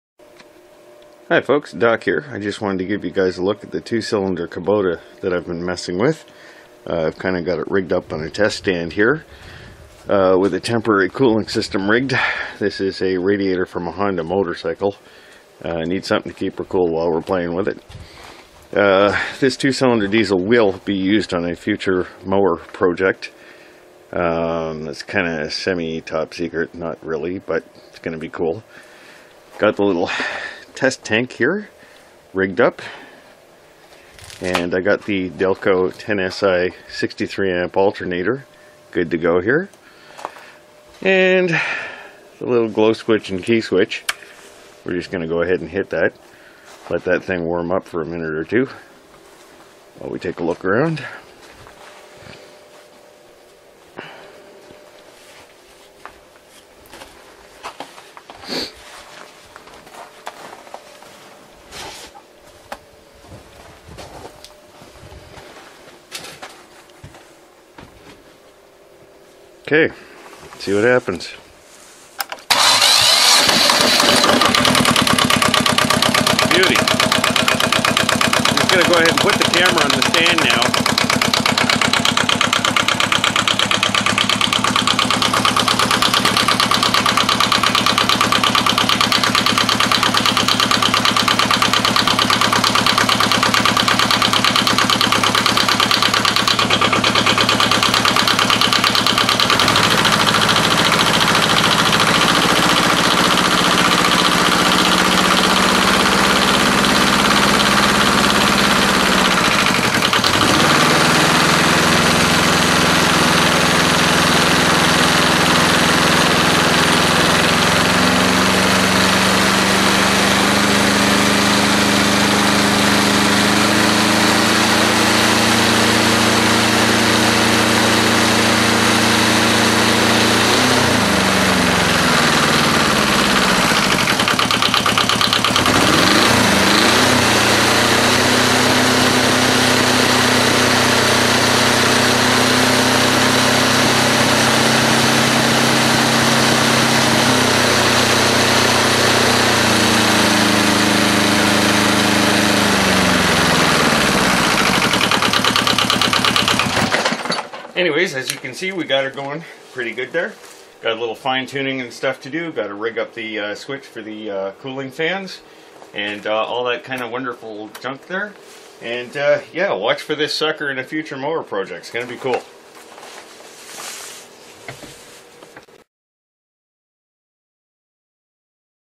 Kubota Z400 Test Run
Got my Kubota 2 cylinder diesel set up on a test stand, with a gravity feed fuel tank made from a stainless steel water bottle. Adapted a GM Delco 10si 63 amp alternator on for charging duty. Cooling is being handled by a radiator from a 1983 Honda Interceptor 750 motorcycle. Ran it for a minute at idle and high speeds. Running great.